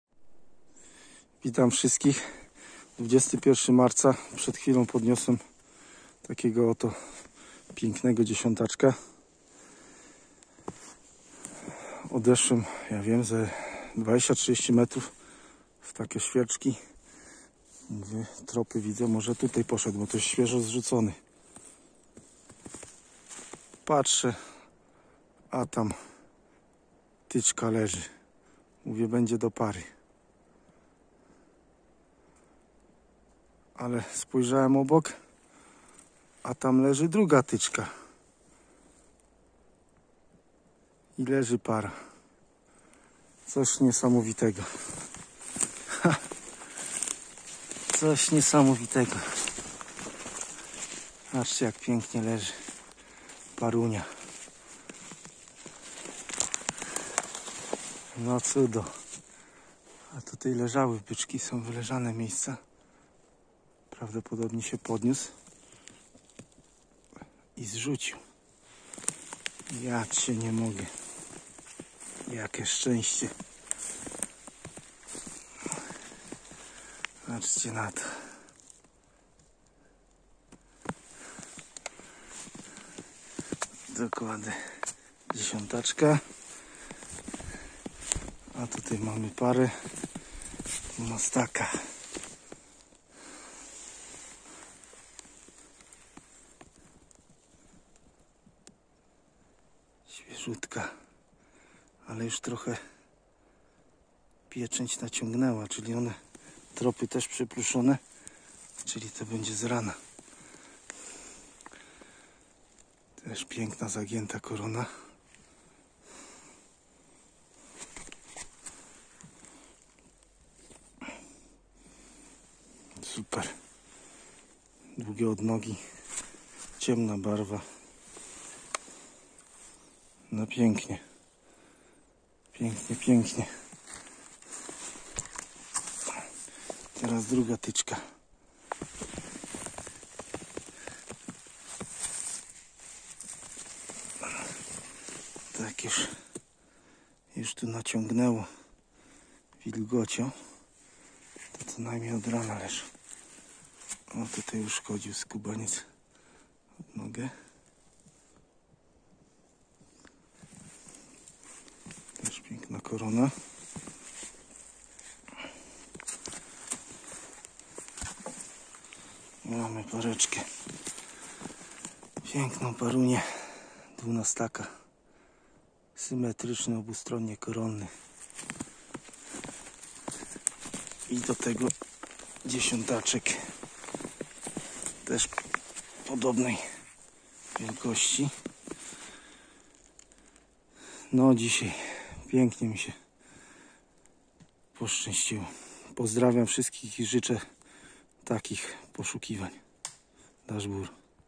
Zrzuty 2021 3 sztuki Para 12-staka i 10-tak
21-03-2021 W tą Niedziela pogoda była brzydka, mocno wiało i co chwilę padał śnieg..:( Ale okazała się dla mnie szczęśliwa:) Najpierw podniosłem 10-taka 1.60 kg a za 5 minut parę 12-staka 3.55 kg. :)